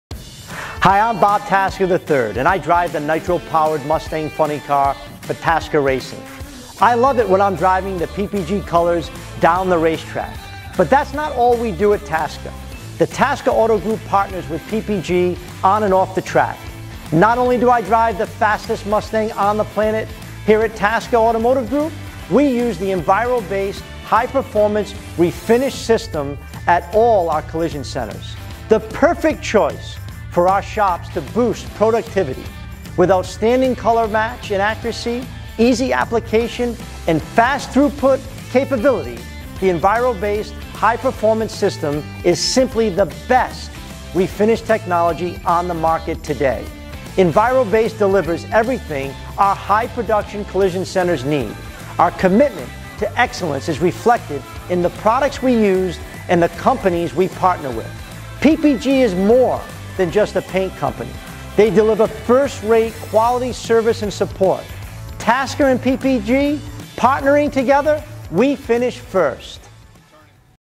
On and Off the Track With Tasca Racing and PPG
PPG and Tasca Racing have a great partnership both on and off the track. Bob Tasca III tells you more about our teamwork in this video.